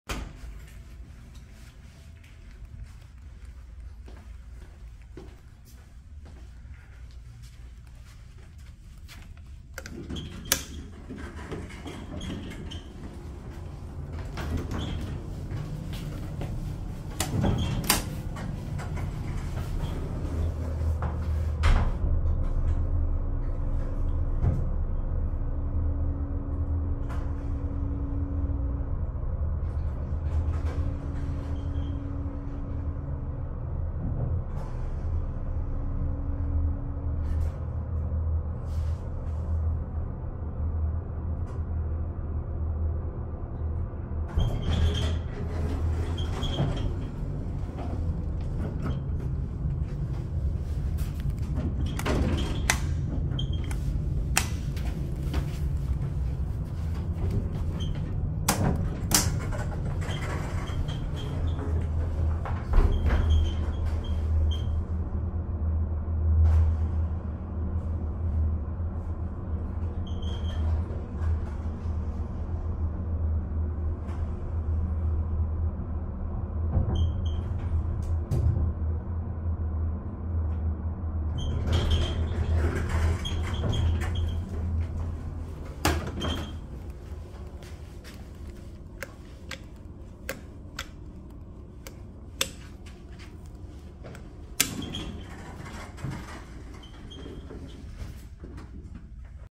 Gallagher And Speck Elevator at 1601 Bond Street, Naperville, IL
This is by Red Roof Inn.